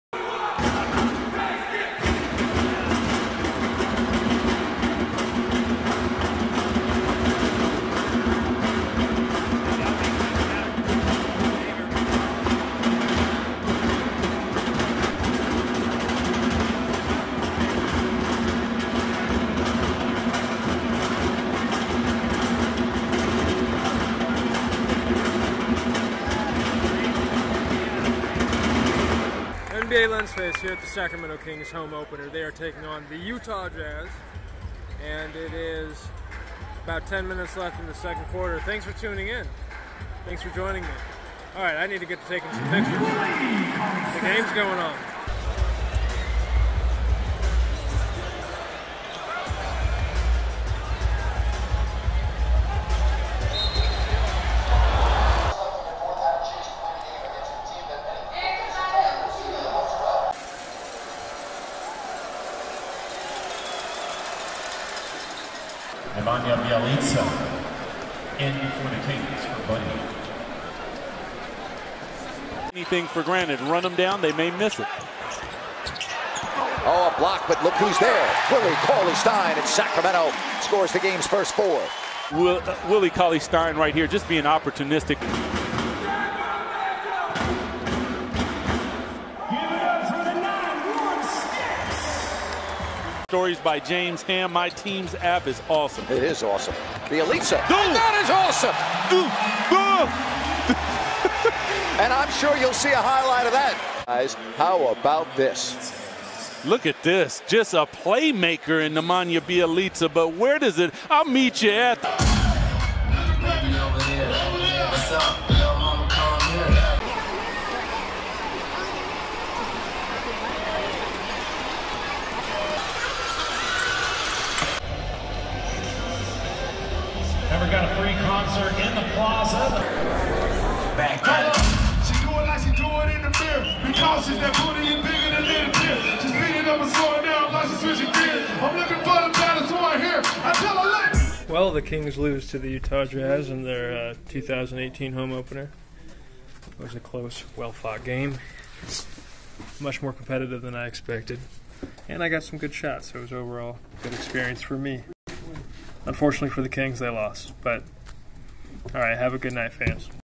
Lensface ep.75 • Oh, What A Night • Sacramento Kings vs Utah Jazz (L, 123-117)
Lensface ep.75. Oh, What A Night
@utahjazz 123, @sacramentokings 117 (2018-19 Season Opener)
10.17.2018
“The first game of the 2018-19 NBA season is officially in the books for the Sacramento Kings. Unfortunately, it resulted in a loss to the Utah Jazz by a score of 123-117, but there was still a lot to enjoy from this one.” From Sanjesh Singh via @sactownroyalty
Lensface is a character performed by Kimani Okearah
This content produced by Let Me Out Productions @letmeoutproductions for @sactownroyalty
Follow @thekimansta or @nbalensface on instagram to follow live!